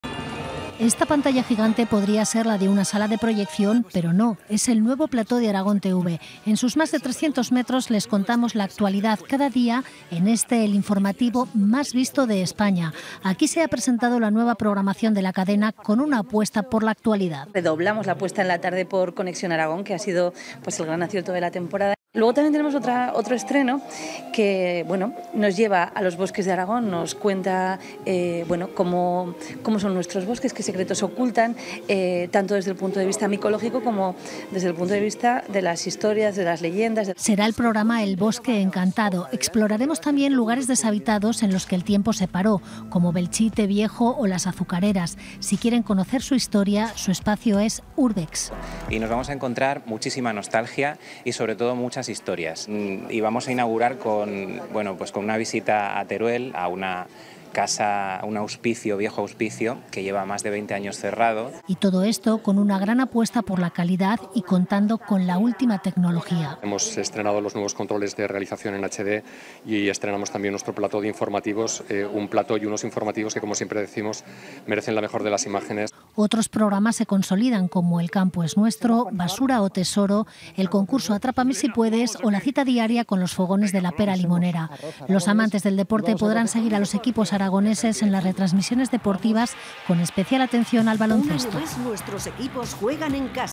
Actualidad, información, deporte y entretenimiento con sello aragonés
La nueva temporada de la televisión aragonesa contará con estrenos como ‘Urbex’ o el ‘Bosque encantado’, amplía el tiempo de ‘Conexión Aragón’ y dedica una noche al género documental

Además, regresarán con nuevos capítulos los programas de más éxito de la cadena como ‘El campo es nuestro’, ‘Basura o Tesoro’, Oregón TV’ o ‘Tempero’